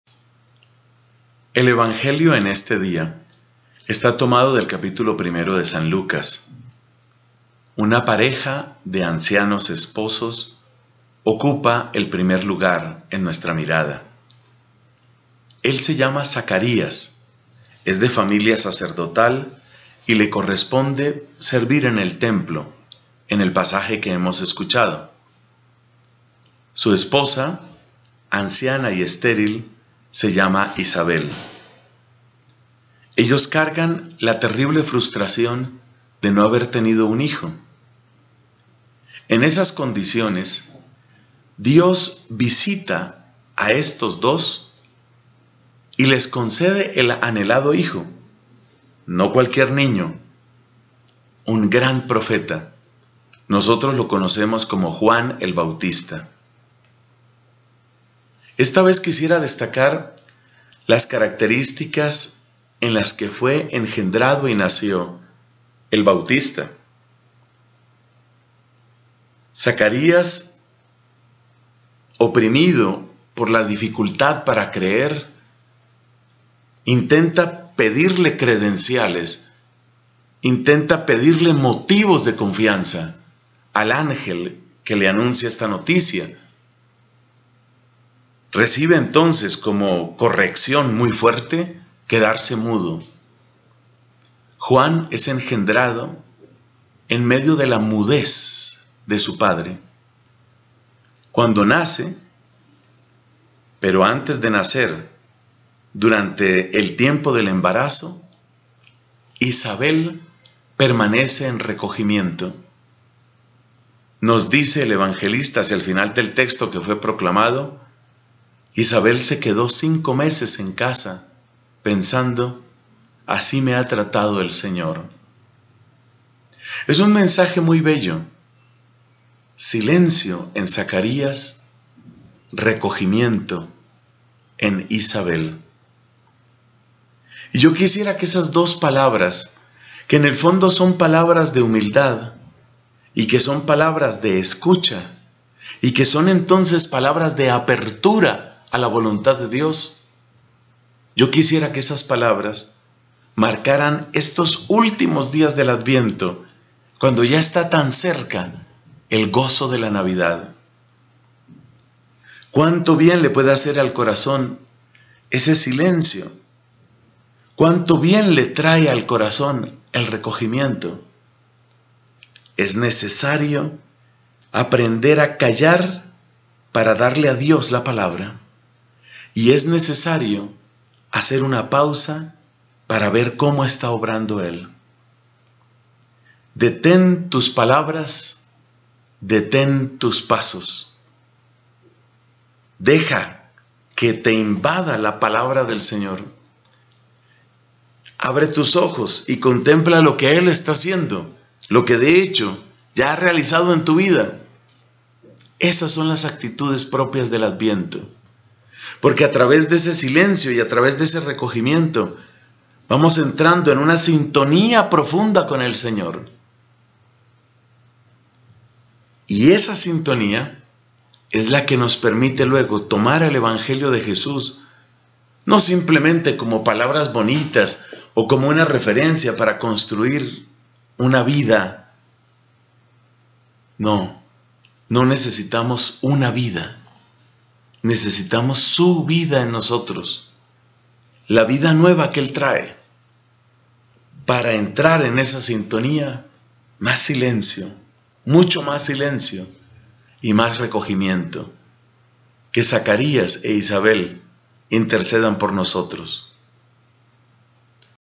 LA GRACIA del Sábado 19 de Diciembre de 2015
Al final de este tiempo de Adviento callemos para que la palabra de Dios nos invada y hagamos una pausa para contemplar lo que ha hecho y está haciendo Él en nosotros.